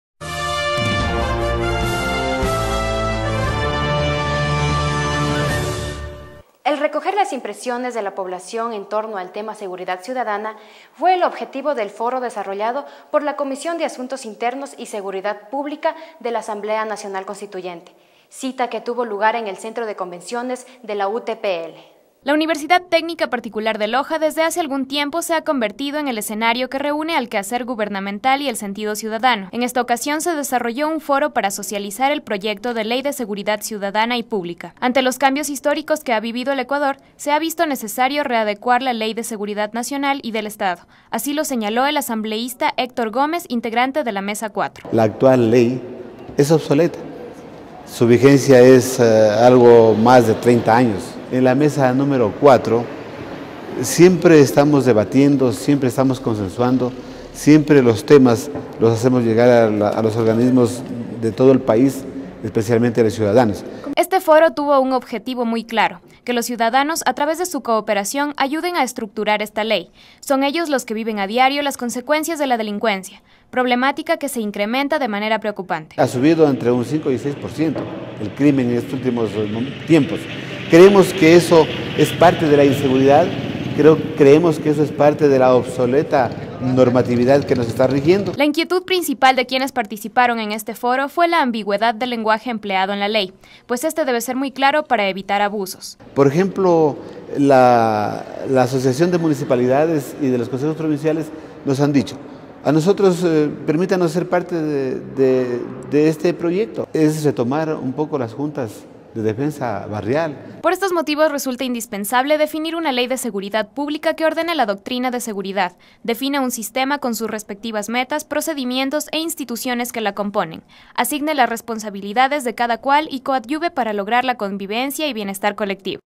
Foro Ciudadano Seguridad Pública
El recoger las impresiones de la población en torno al tema seguridad ciudadana fue el objetivo del Foro desarrollado por la Comisión de Asuntos Internos y Seguridad Pública de la Asamblea Nacional Constituyente, cita que tuvo lugar en el Centro de Convenciones de la UTPL.